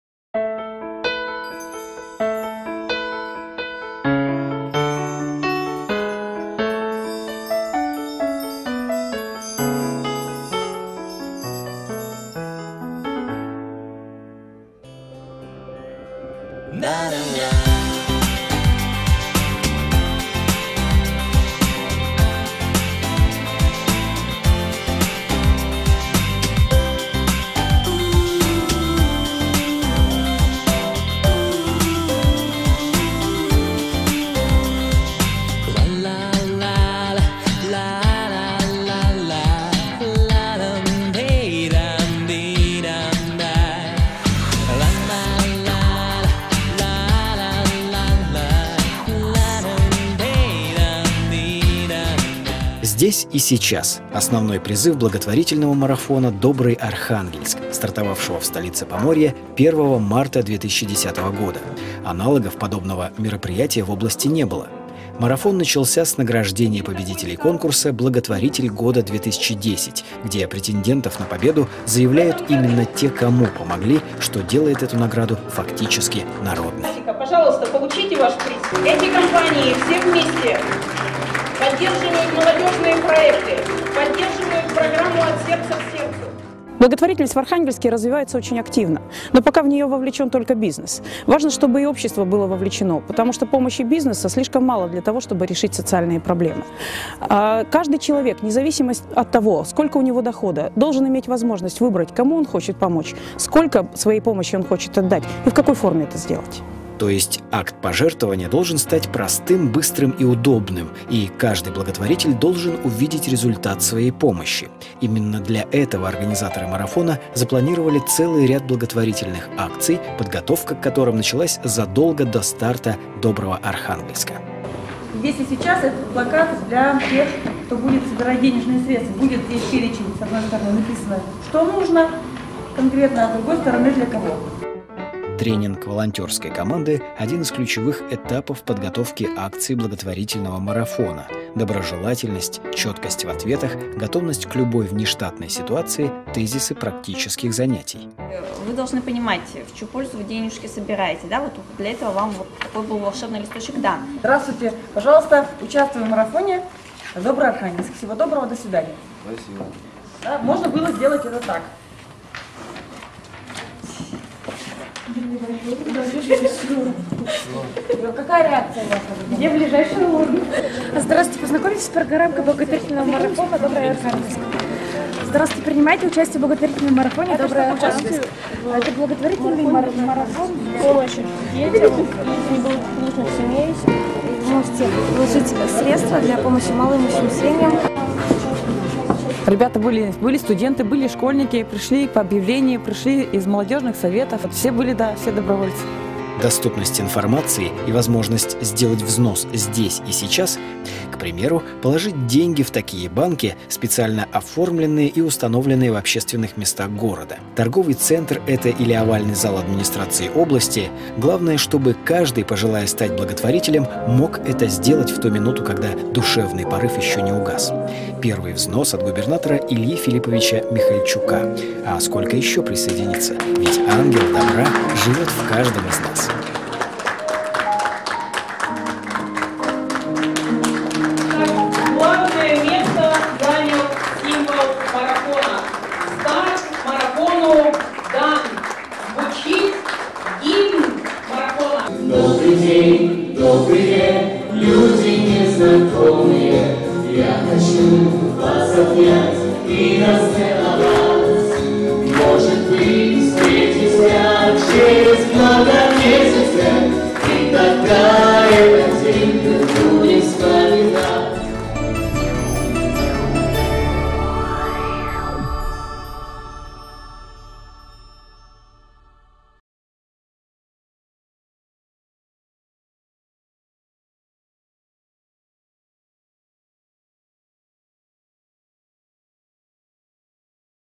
Благотворительный марафон "Добрый Архангельск" часть 1
Презентация состоялась в рамках Инновационной лаборатории 22 августа 2016 г.